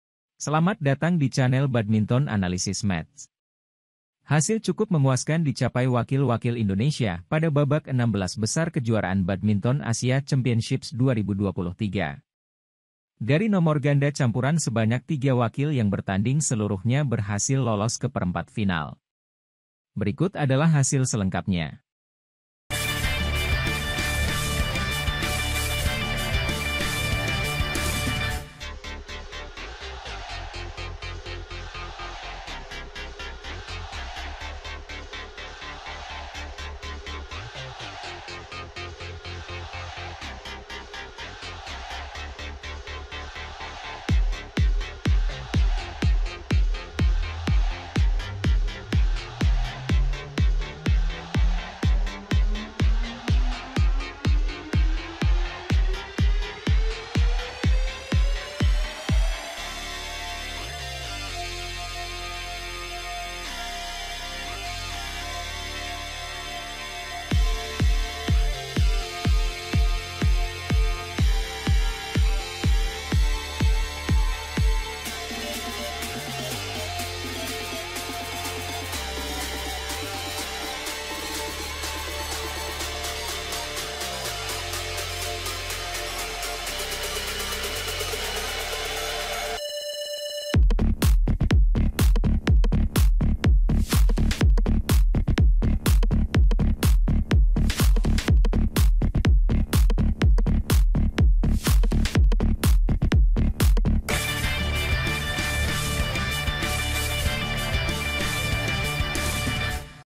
Hasil BAC 2023 Babak 16 Besar : Praveen/Melati, Dejan/Gloria, Rinov/Pitha Tembus Quarter Final.
Hasil BAC 2023 babak 16 besar. Praveen/Melati, Dejan/Gloria, Rinov/Pitha, Gregoria Mariska, Bagas/Fikri dan Febriana/Amalia berhasil lolos ke babak Quarter Final.